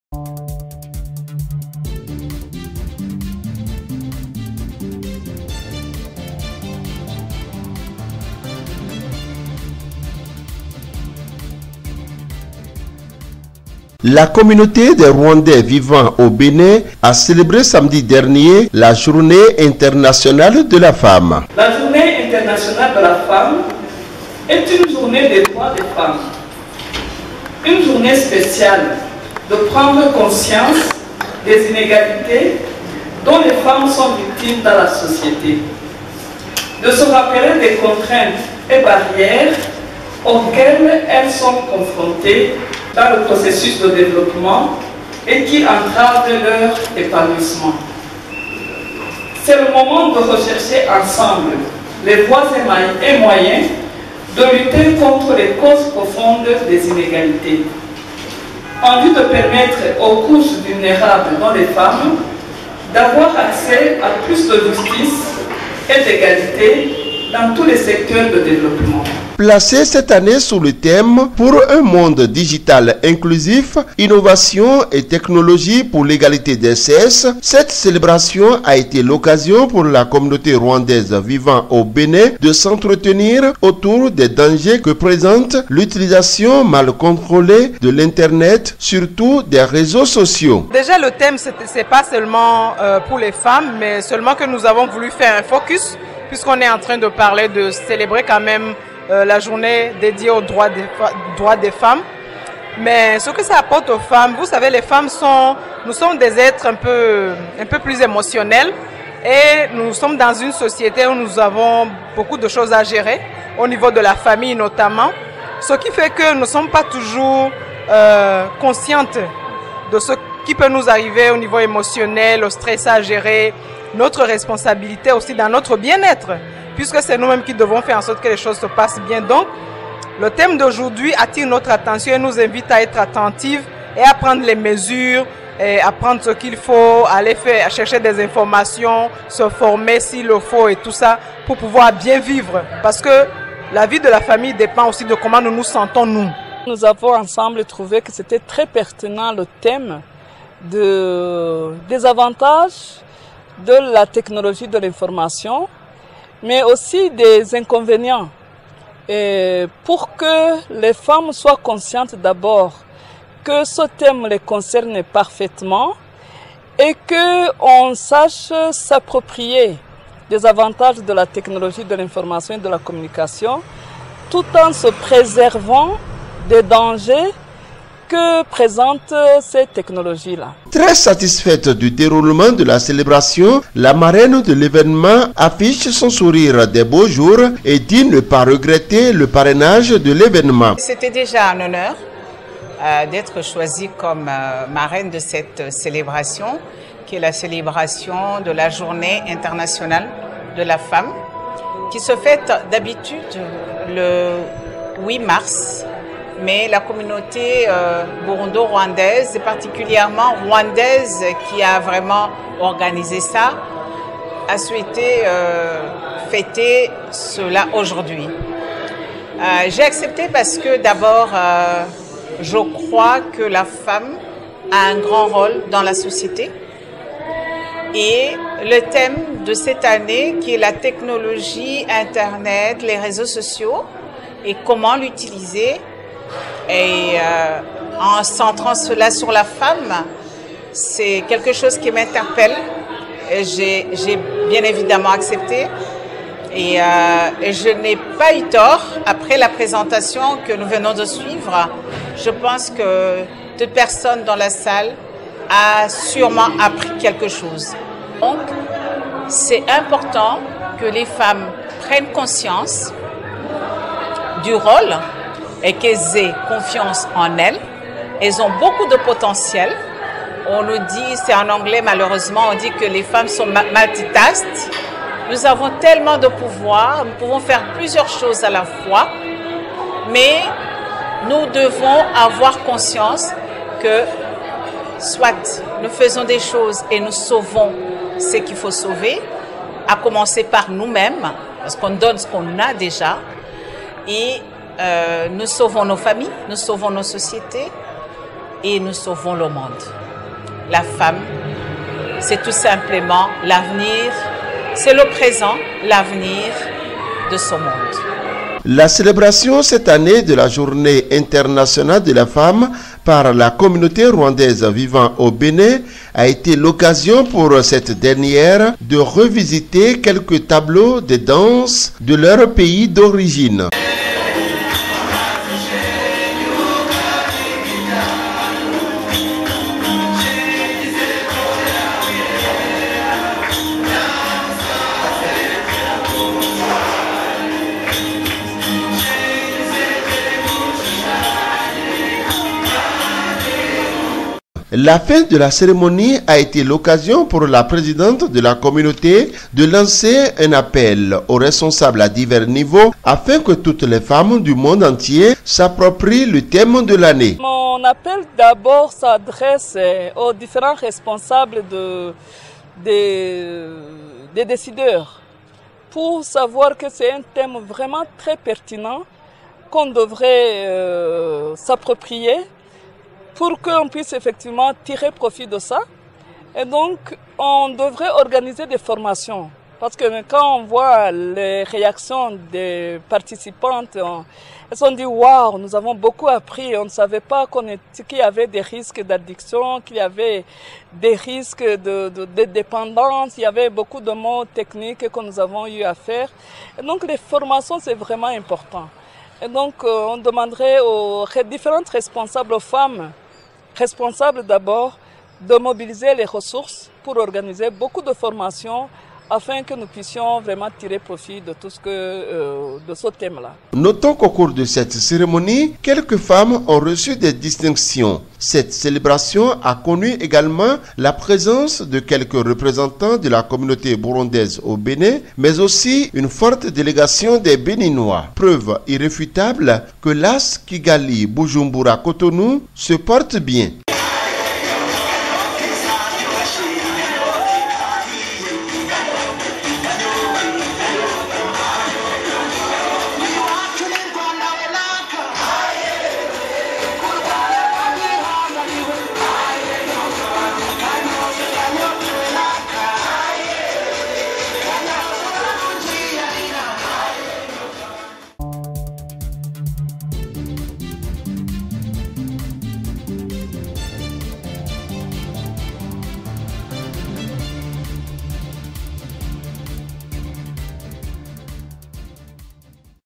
💗JOURNÉE INTERNATIONALE DE LA FEMME 2023
🇷🇼 La communauté des Rwandais vivant au Bénin célèbre l'événement à Cotonou.